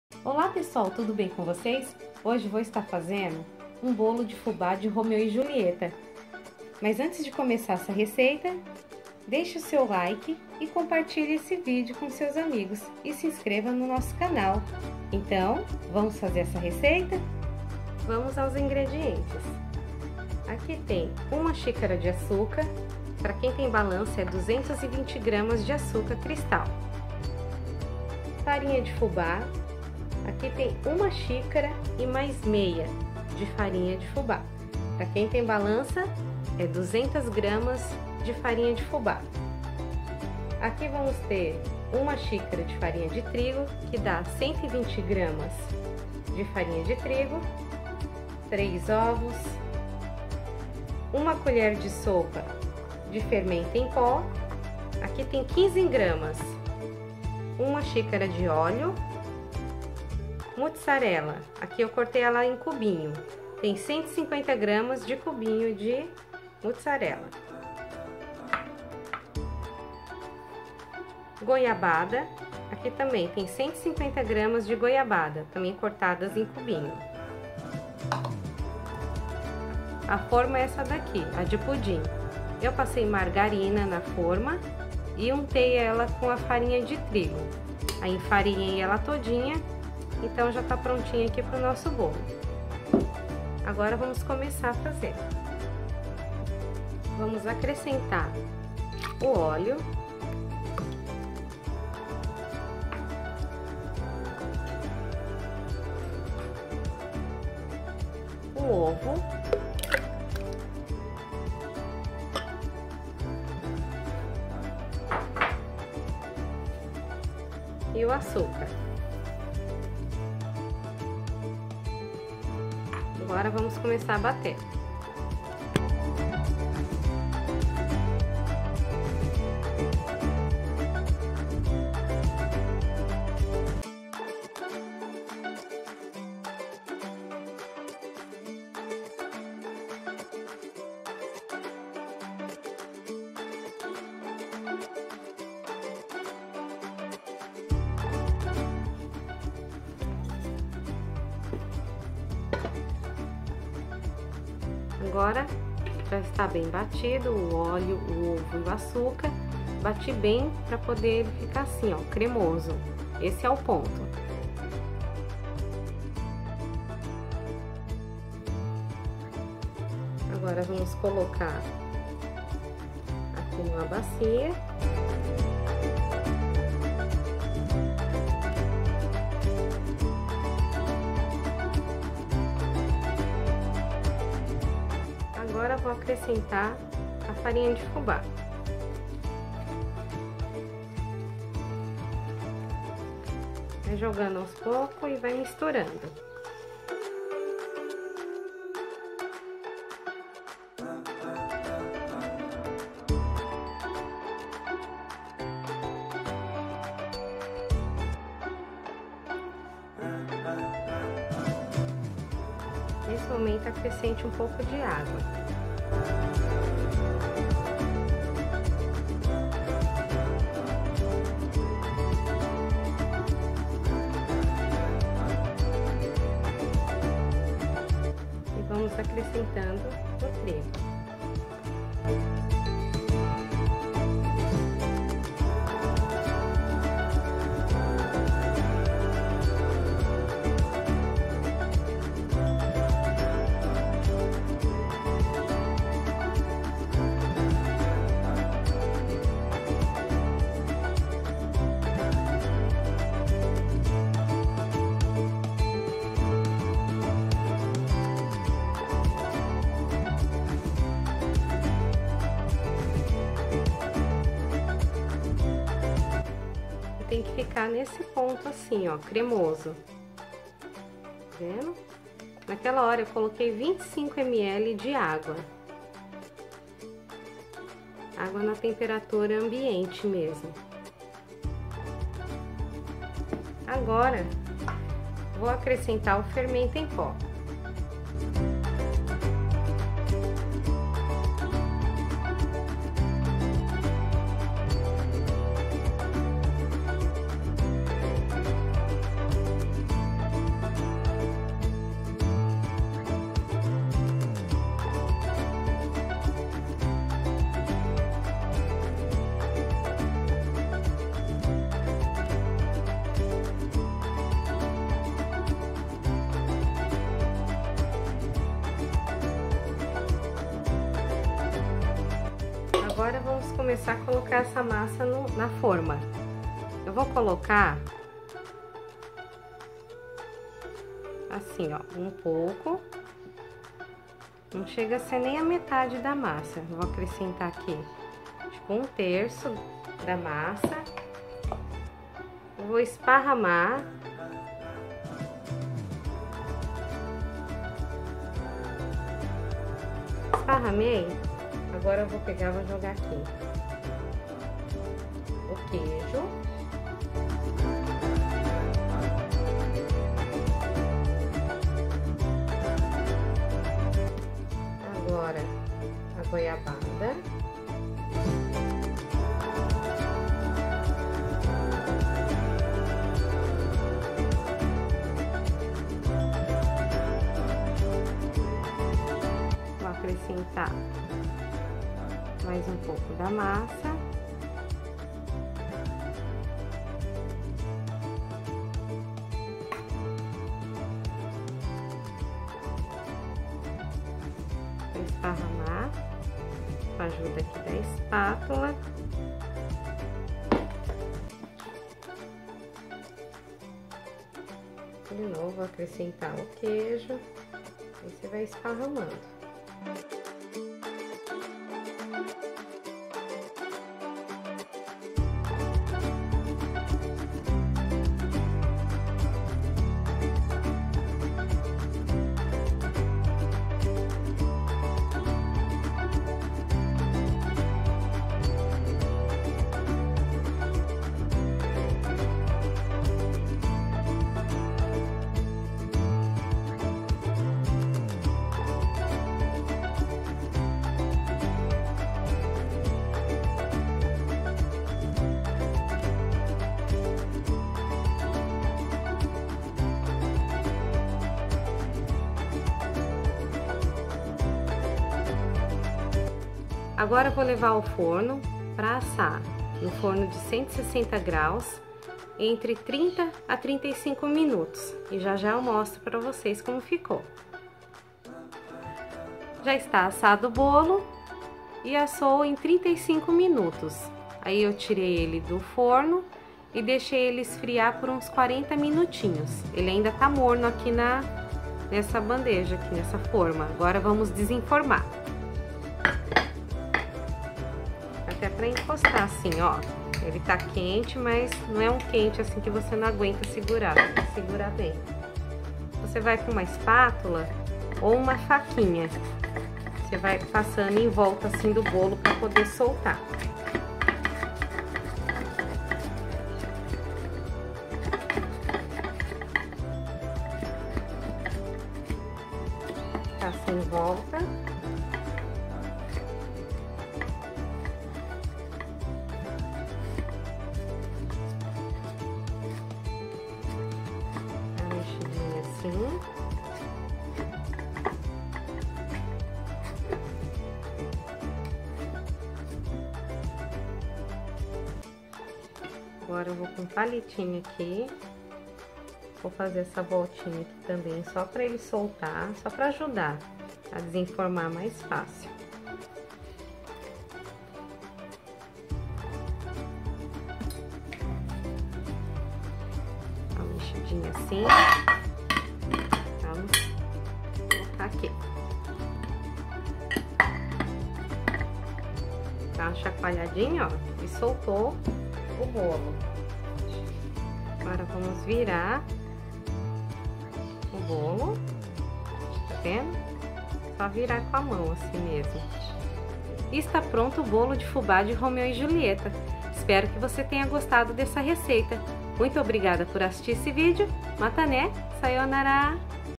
Bolo de fubá romeu para festa junina / bolo de liquidificador/
Esse bolo é muito fácil de fazer, macio e gostoso, gosto de fazer também sem o queijo e a goiabada e fica maravilhoso, quentinho com uma margarina e um café fica perfeito, faça pra sua família .

Bolo de fubá de romeu e julieta

1 xícara ou 220g de açucar cristal
1 1/2 xícara ou 200g de farinha de fubá
1 xícara ou 120g de farinha de trigo
3 ovos
1 colher (sopa) ou 15g de fermento
1 xícara de óleo
25ml de água
150g de mussarela
150g de goiabada

Nos siga também no instagram e no facebook